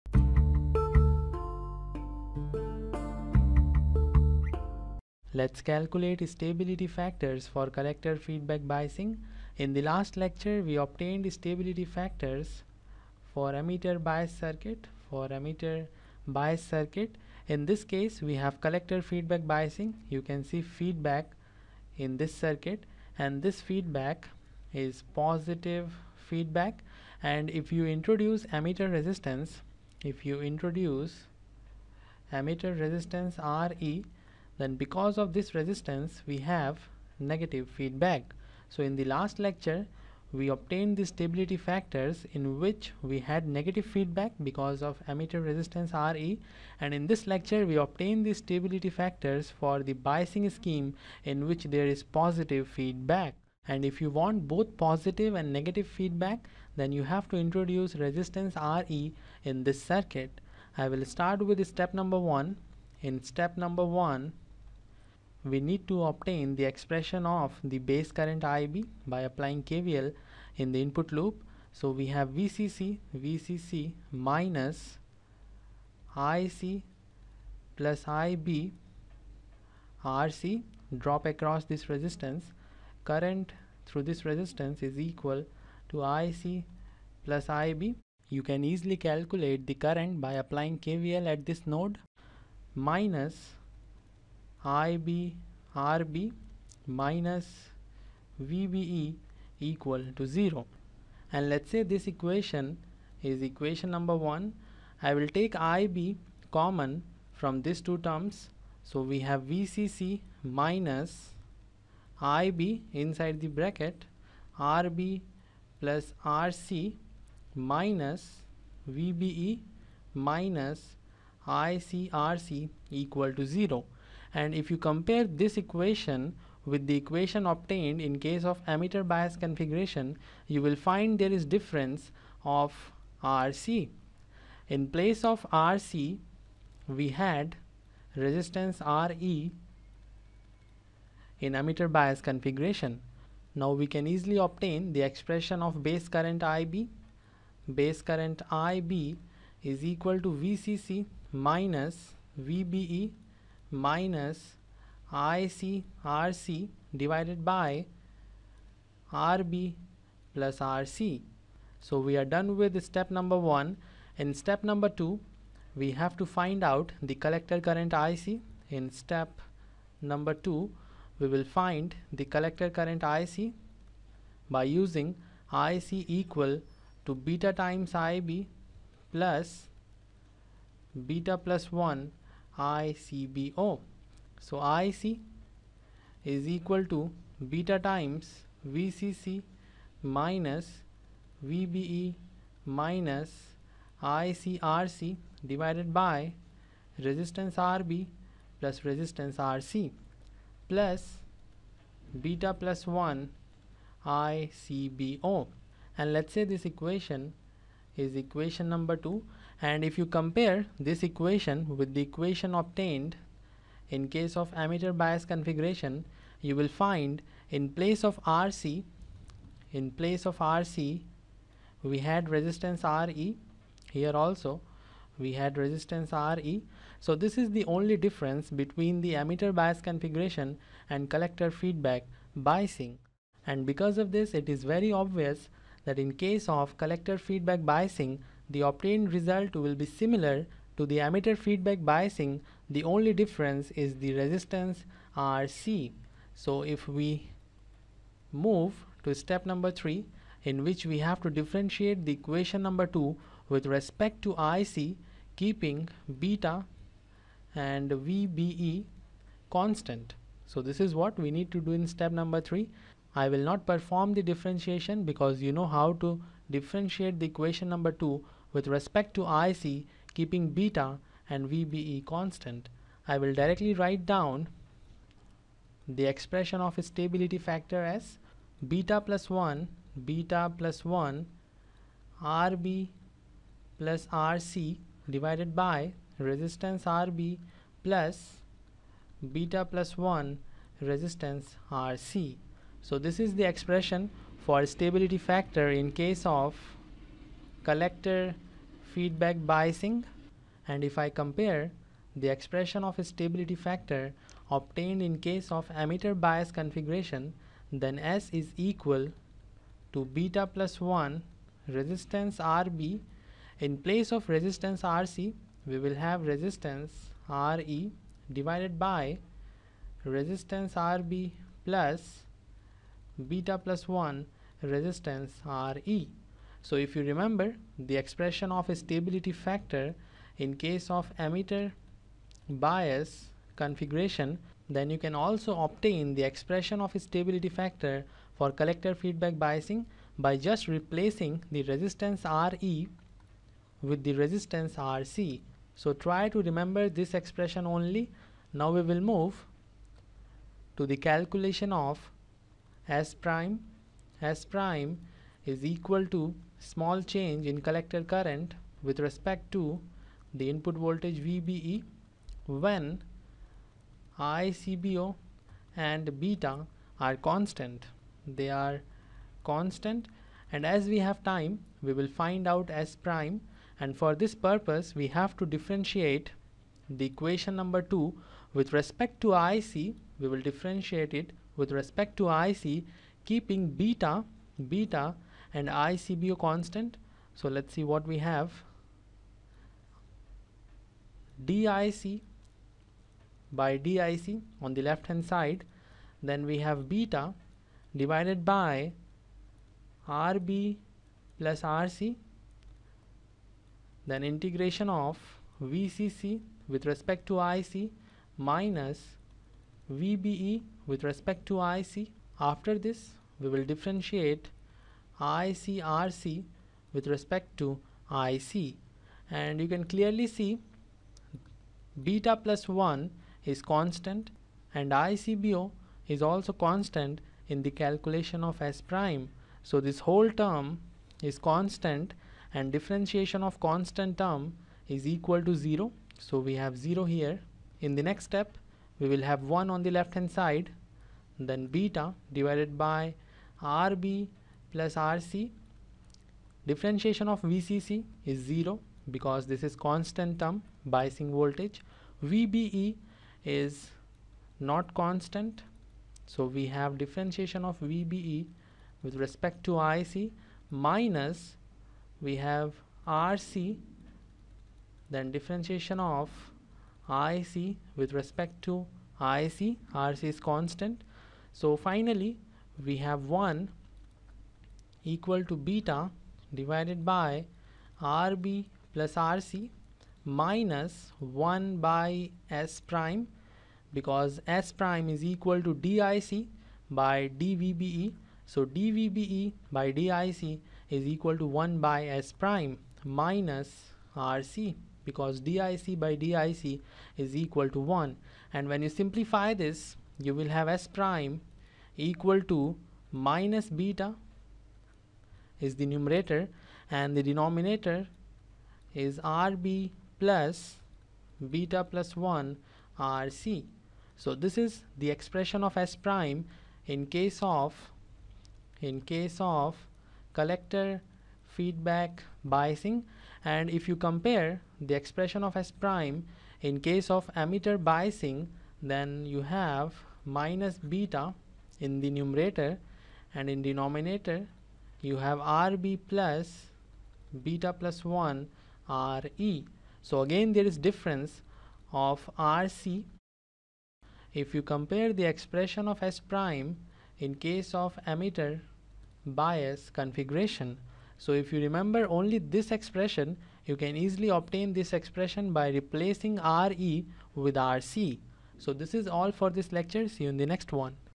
Stability Factor for Collector Feedback Biasing
Analog Electronics: Stability Factor for Collector Feedback Biasing
Topics Covered:
1. Calculation of stability factors for collector feedback bias.